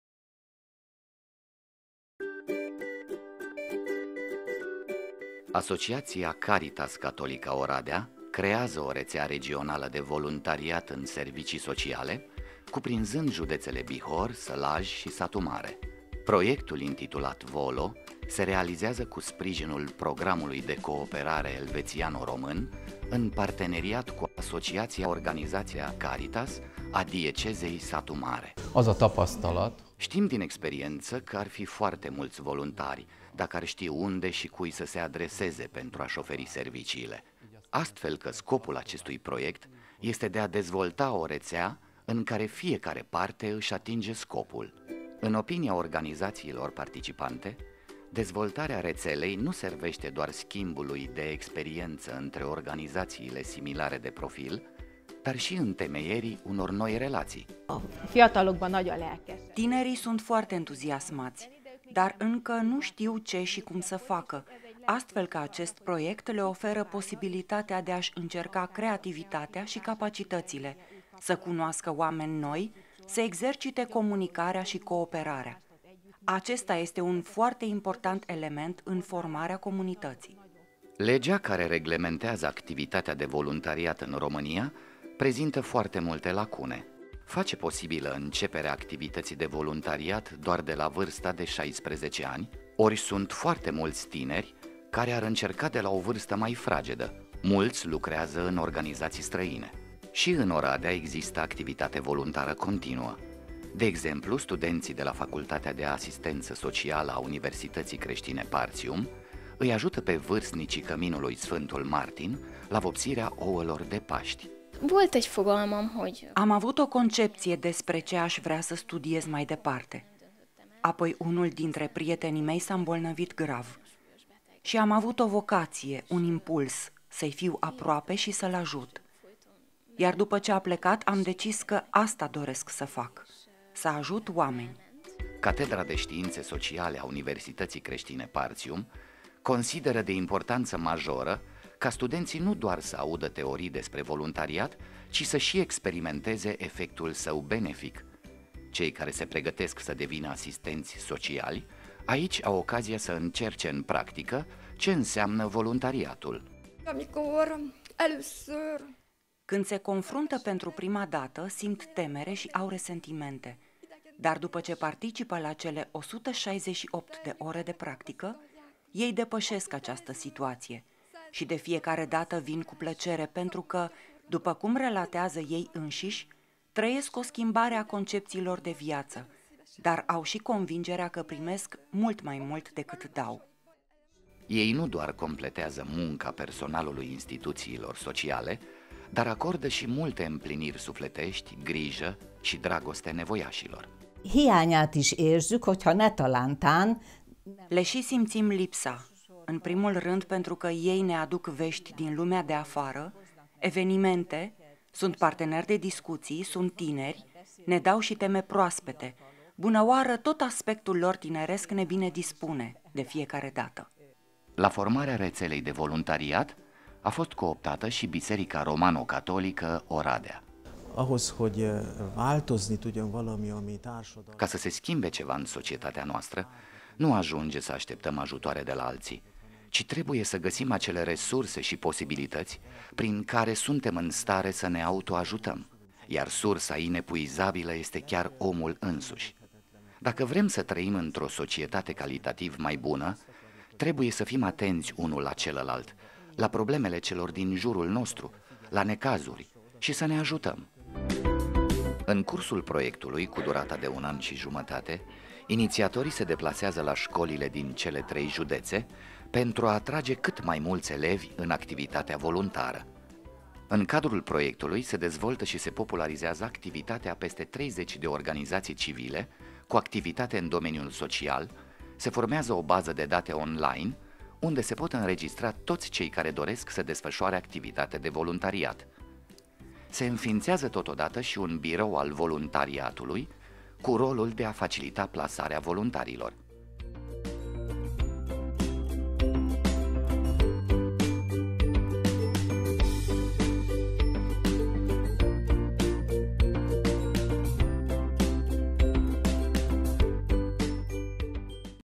VOLO - Rețea regională de voluntariat Bihor - Satu Mare - Sălaj
Rețeaua regională de voluntariat în servicii sociale, în județele Bihor, Satu-Mare și Sălaj VOLO este o bază de date a voluntarilor, creată de Caritas Catolica Oradea în vederea corelării cererii și ofertei în domeniul serviciilor sociale. Organizațiile civile își pot posta proiectele care necesită voluntari, iar voluntarii înregistrați pot căuta printre proiecte.

Proiectul este co-finanțat printr-un grant din partea Elveției prin intermediul Contribuției Elvețiene pentru Uniunea Europeană extinsă.